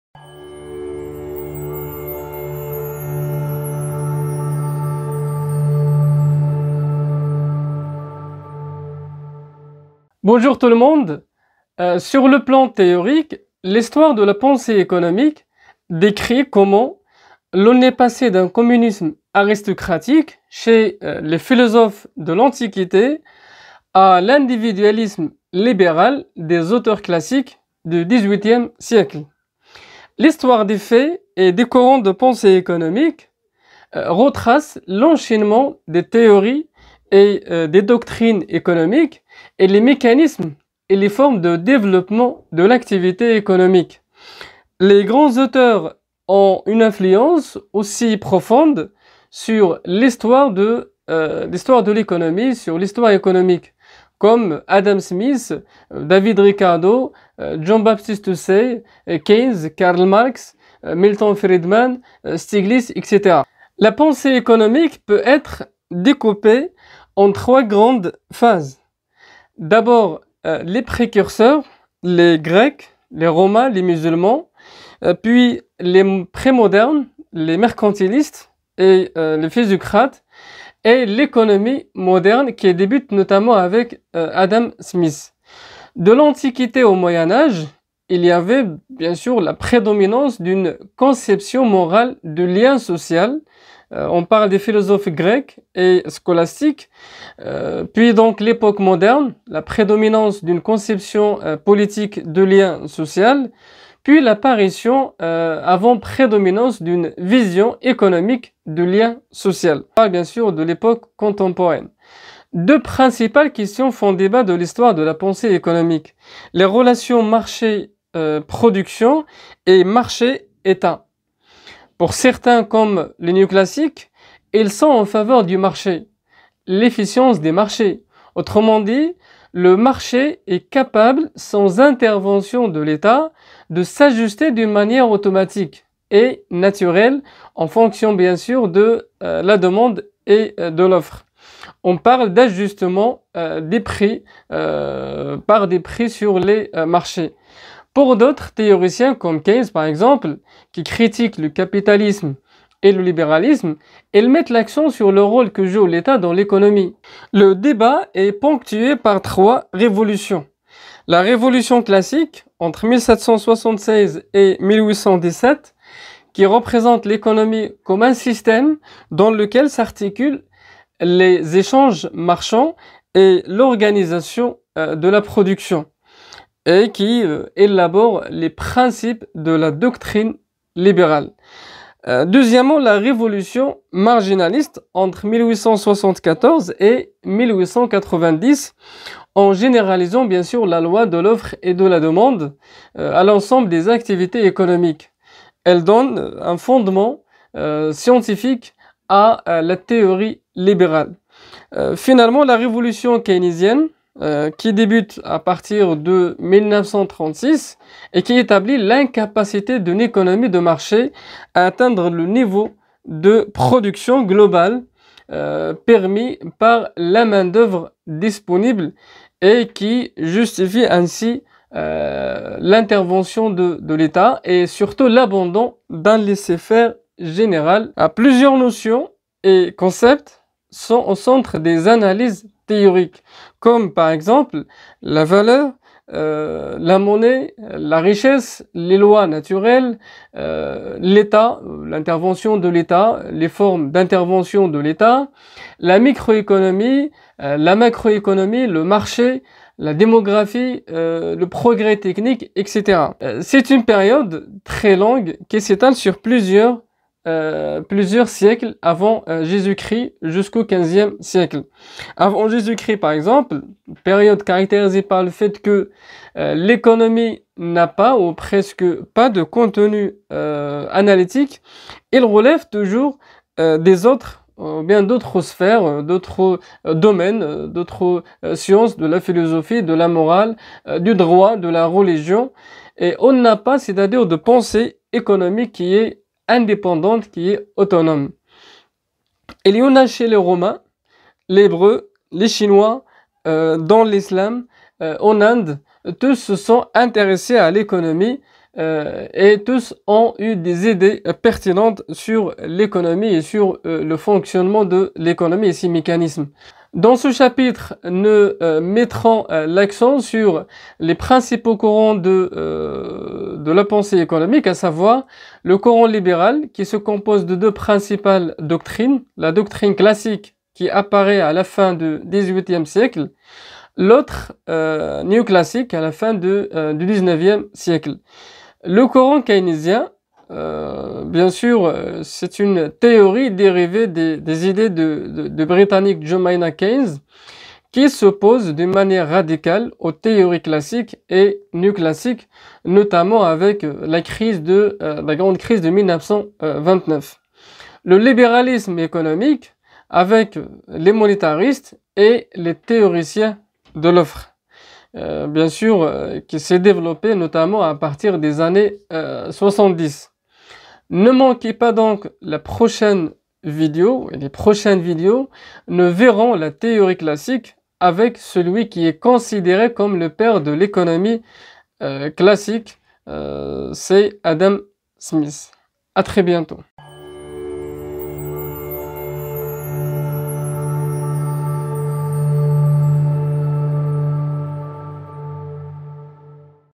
Les courants de la pensée économique. Présentation générale.
Chapitres
0:00 Introduction
1:04 Quelles sont les 3 grandes phases de l'histoire de la pensée économique et les 2 questions qui font débat ?
4:13 Quel est l'origine des systèmes de pensées économiques ?

La pensée économique peut être découpée en trois grandes phases : les précurseurs (grecs, romains, musulmans), les pré-modernes (Mercantilistes, Physiocrates) et l’économie moderne (qui débute avec Adam Smith).
Les grands auteurs ont une influence aussi profonde sur l’histoire économique, comme Adam Smith, David Ricardo, JB Say, JM Keynes, Karl Marx, Milton Friedman,  Joseph Stigliz.
Deux principales questions font débat de l’histoire de la pensée économique : Les relations marché/production et marché/Etat.
Dans ce chapitre, nous mettrons l’accent sur les principaux courants à savoir : Le courant libéral (classique et néoclassique), le courant keynésien et le libéralisme économique.